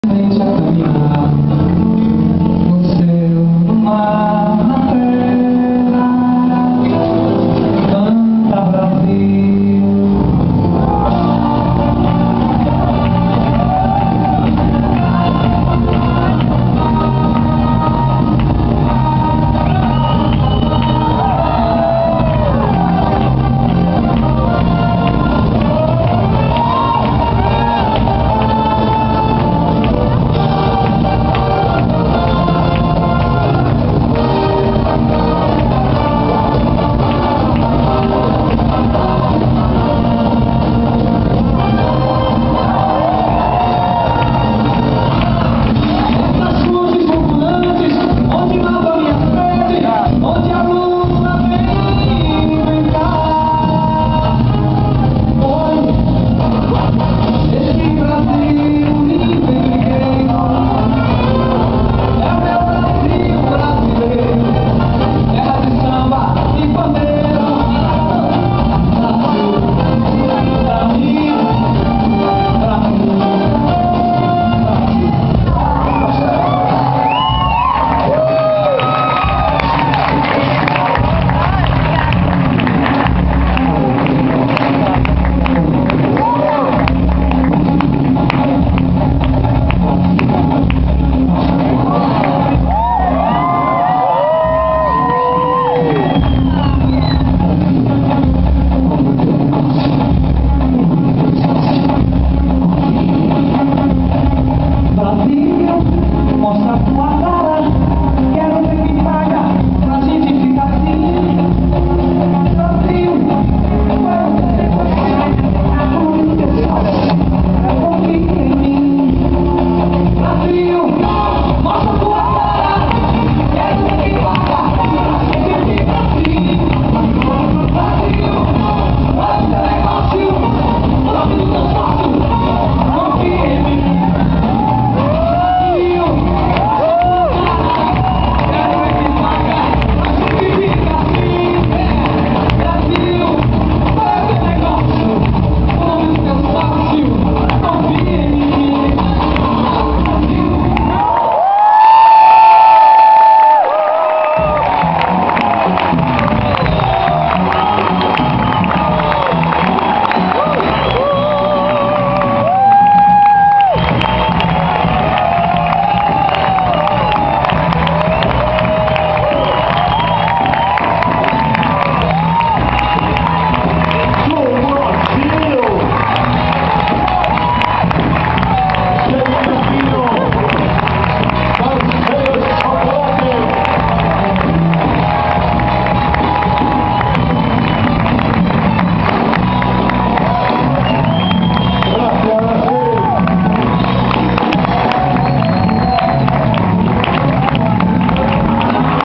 Samba de gafieira - Marcelo Chocolate e Sheila Aquino
Apresentação em La Viruta (Buenos Aires, Argentina) em março de 2010.